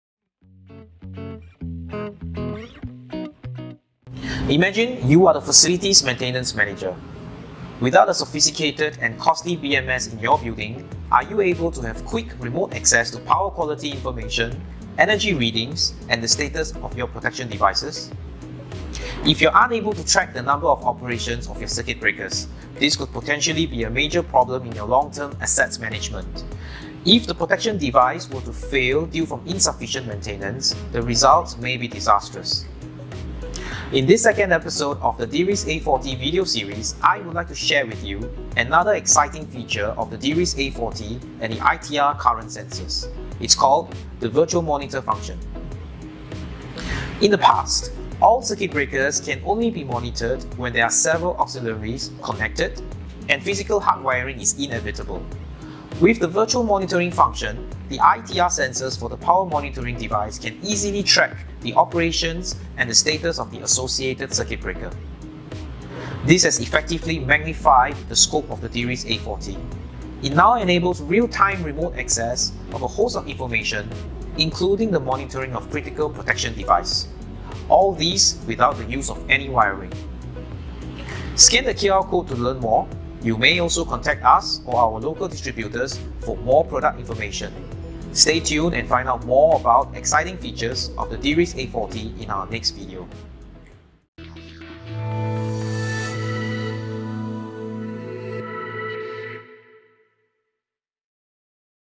DIRIS A40 Virtual Monitor
Ways to monitor the status of your circuit breakers without adding several accessories with massive wiring works.
With the DIRIS A40, you can now do it effortlessly and be thrilled with the features of the integrated webserver built-in.